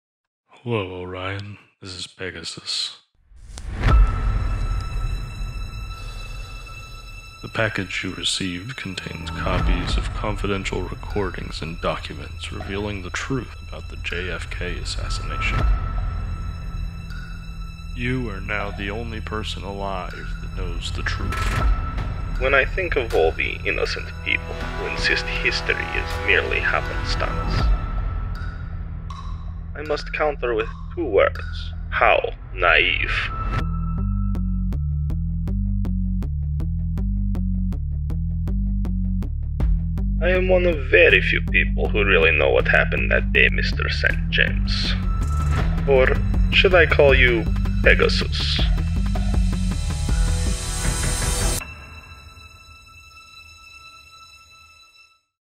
Pegasus Directive book trailer
THE PEGASUS DIRECTIVE: The Top Secret Kennedy Assassination File (A Justin Scott Thriller Book 3)

"A meticulously detailed, gripping tale that will especially appeal to political thriller fans." - Kirkus Reviews

Year after year, the world’s three most frequently asked questions continue to be:
1. Who killed President John F. Kennedy?
2. Why was he killed?
3. How was it accomplished?
The answers came in May 1972 from a Soviet spymaster seeking sanctuary at the Canadian Embassy in Rome. He told the ambassador at the time he wished to defect to the United States in exchange for perishable information he deemed critical to America’s continued well-being. There was just one caveat: He would only surrender to an American agent he knew solely by the code name Pegasus. What he revealed to Pegasus in trade for asylum was deemed so explosive by President Nixon, that it has remained among the world’s most closely guarded secrets—until now.

The Pegasus Directive is an international thriller released in June 2023 to coincide with the 60th anniversary year of President John F. Kennedy’s assassination.